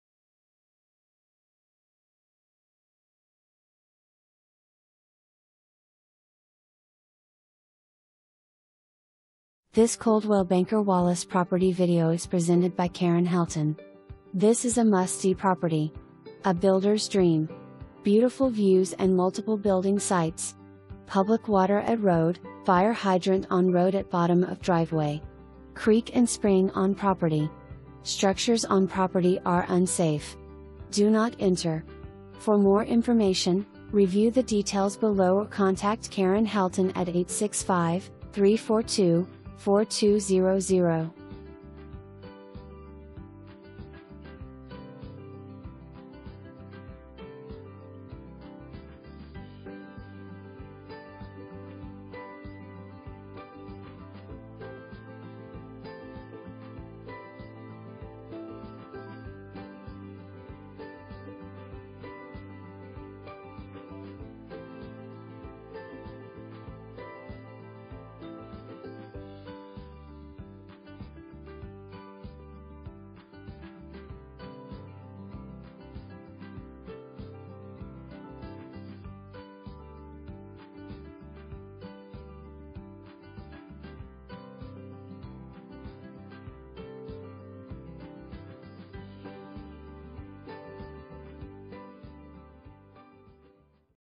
Coldwell Banker Wallace - 941 Old Piney Rd
941 Old Piney Rd
Maryville 37803

This is a must see property!! A builder's dream. Beautiful views and multiple building sites!! Public water at road, Fire hydrant on road at bottom of driveway. Creek and Spring on Property. Structures on property are unsafe. Do not enter.

Karen Helton
Coldwell Banker Wallace